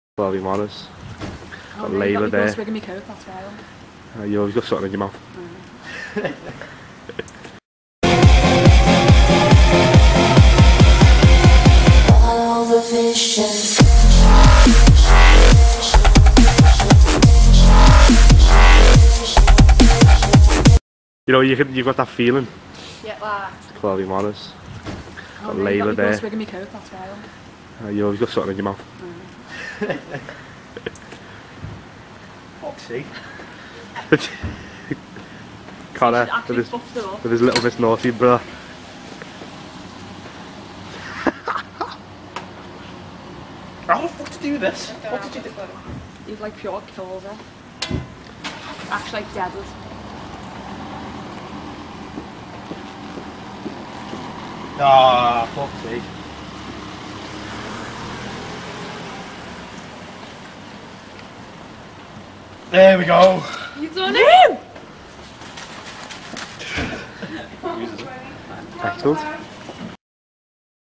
Broken Umbrella
Connor cant seem to get his little miss naughty umbrella to go down properly for some reason, which can be dangerous with the weather we have! CommentLinkSubscribe CLS!